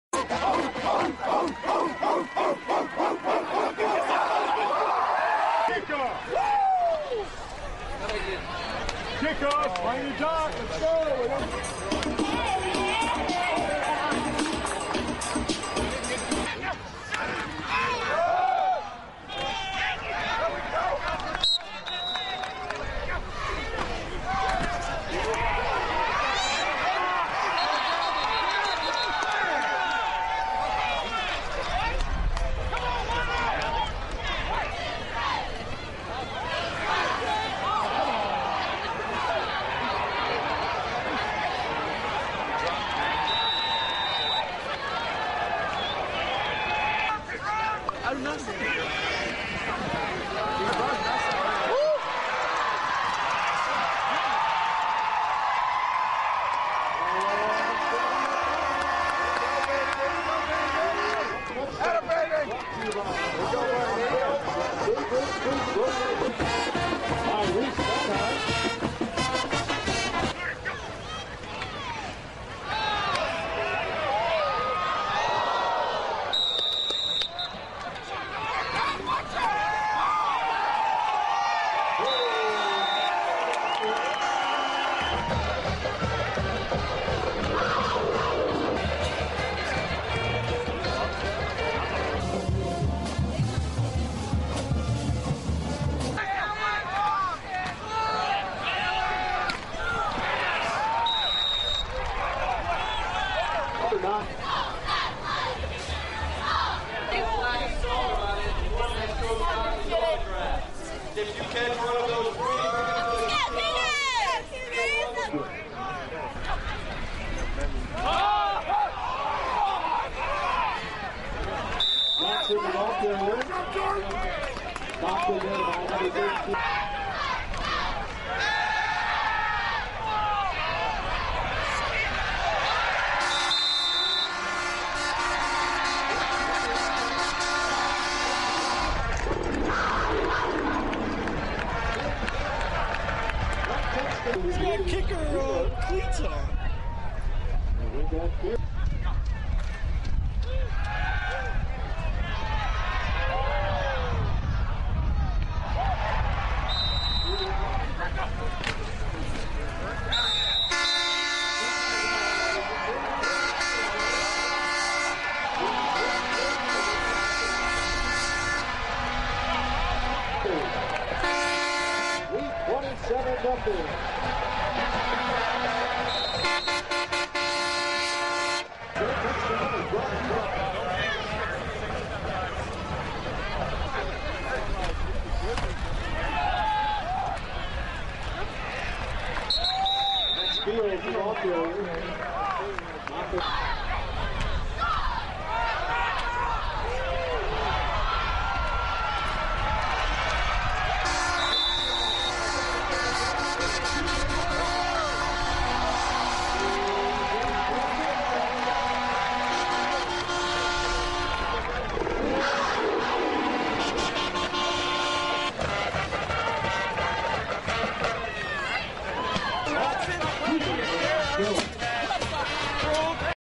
Saint Francis pummels Trinity Int'l, 63-7
The Saint Francis football team remains undefeated on the season after a 63-7 pummeling of Trinity International on Saturday.